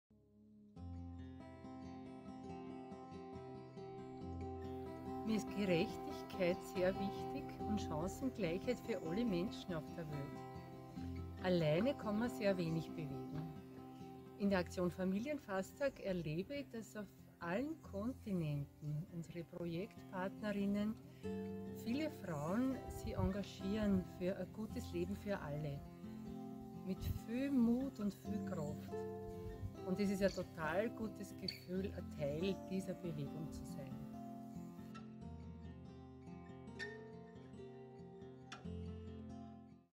Traude Reinwein, Mitglied der kfb Wien
Kfb Mitglieder über die persönliche Bedeutung ihrer Arbeit und die wertvolle Partner:innenschaft von Frauen aus Österreich und Frauen aus dem globalen Süden.